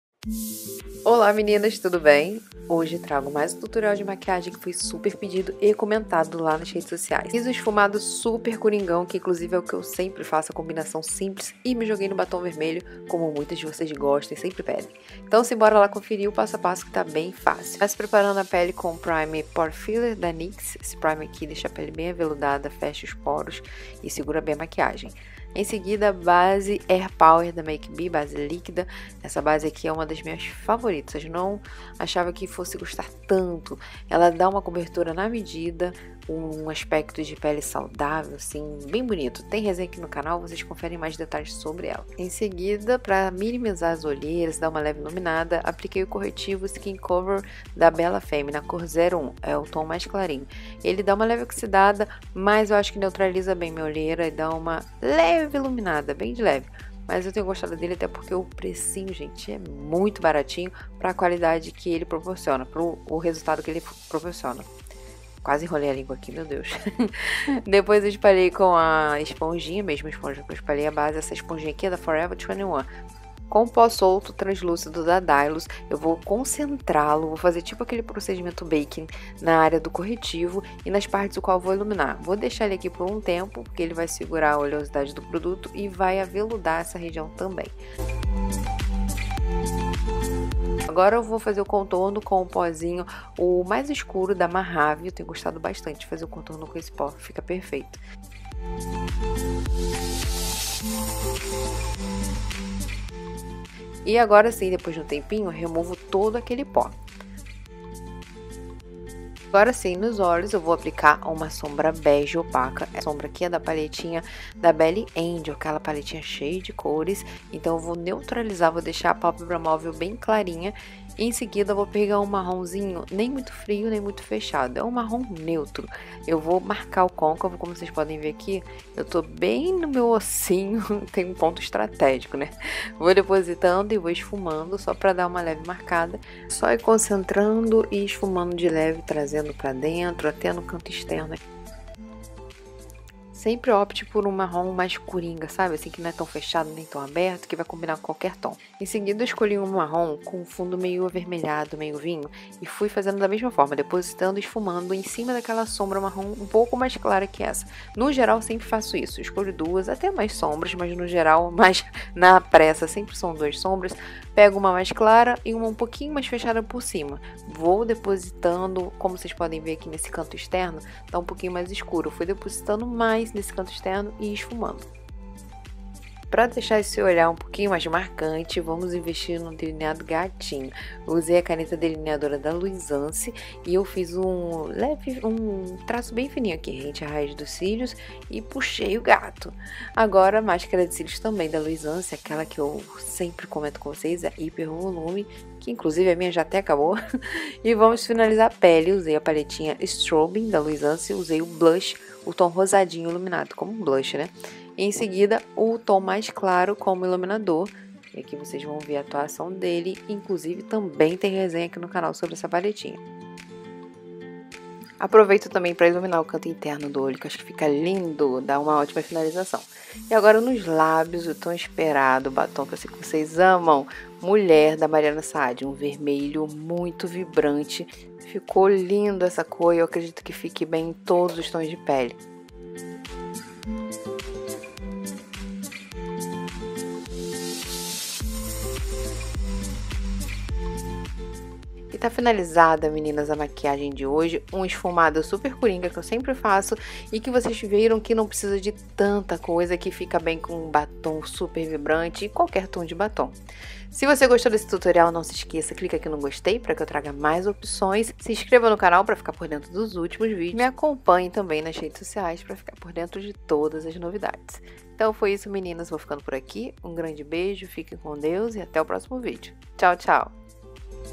Esfumado sem erro 😉
Olá meninas! Maquiagem de hoje vai para as meninas que, assim como eu, adoram uma maquiagem fácil :D
No tutorial de hoje compartilho um esfumado super fácil, rápido e que super combina com aquele batom super tcham :D

Simbora conferir e espero que vocês gostem
bjsss